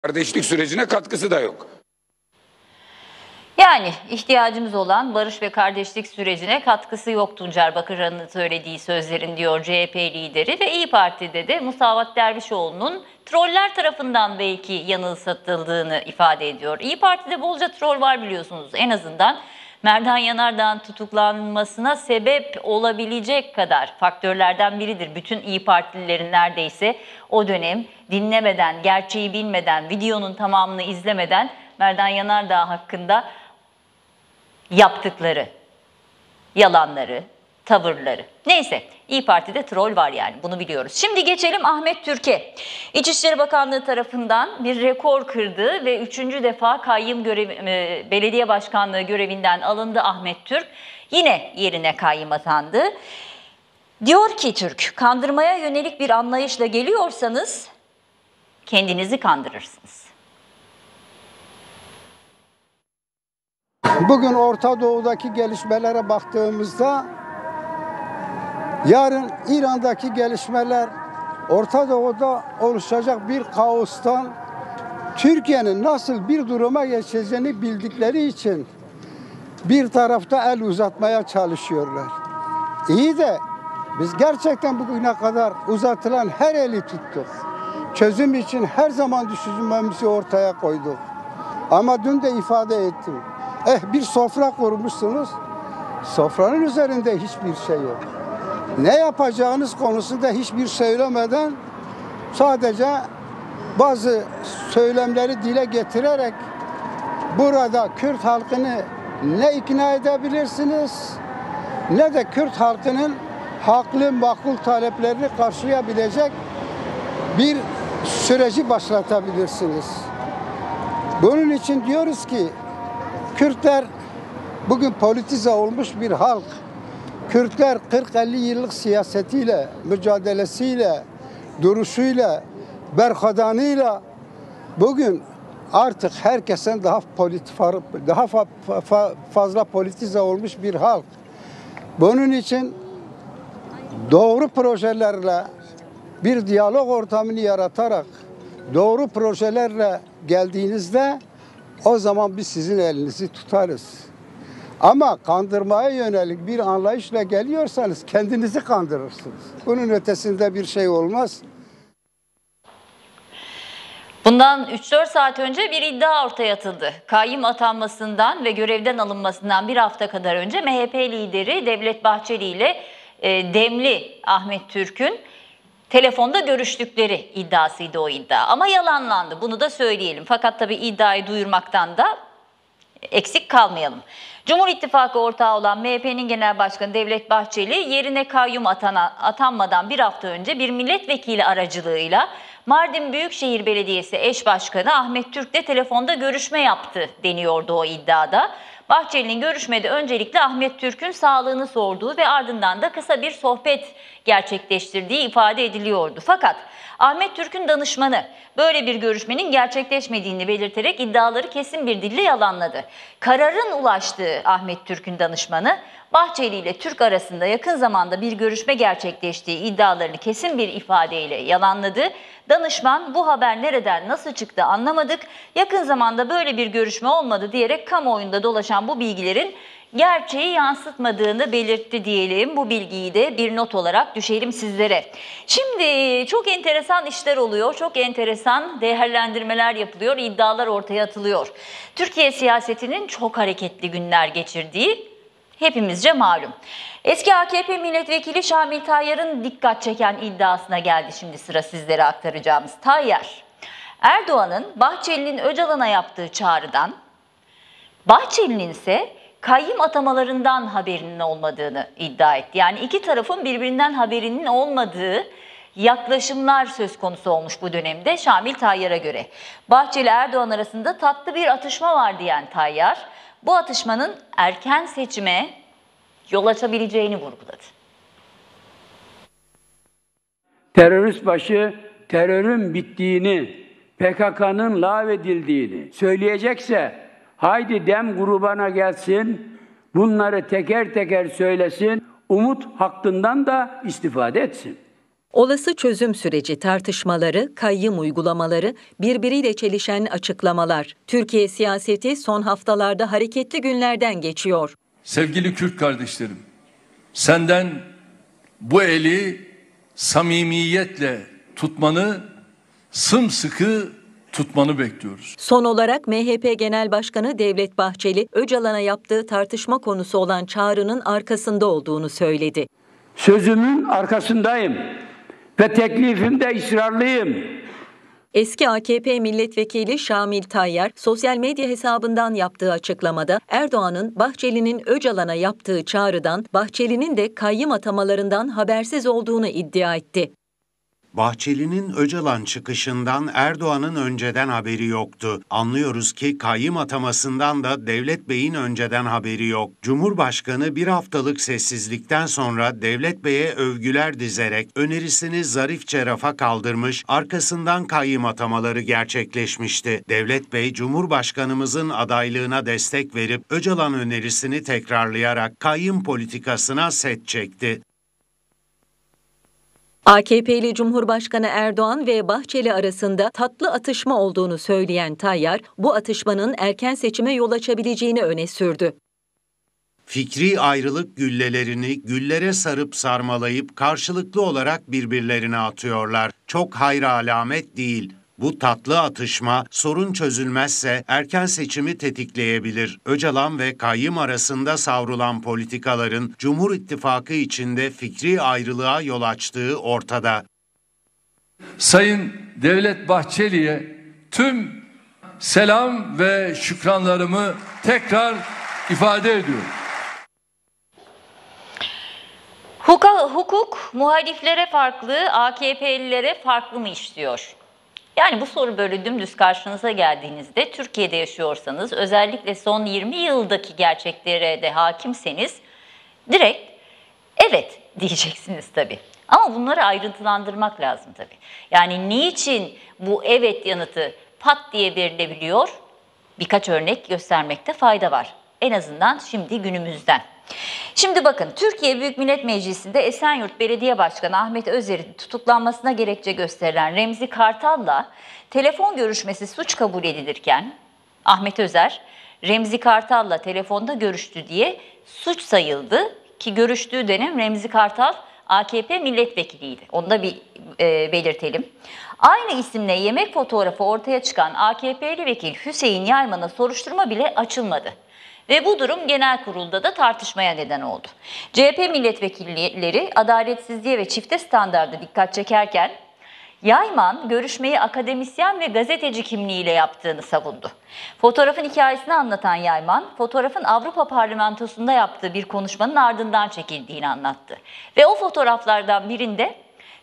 Ahmet Türk'ten Erdoğan ve Bahçeli'ye: Kendinizi kandırırsınız!
Yerine kayyım atanan Mardin Büyükşehir Belediye Başkanı Ahmet Türk, MHP lideri Devlet Bahçeli’nin CHP’nin kendisini istismar ettiği söylemine yanıt verdi.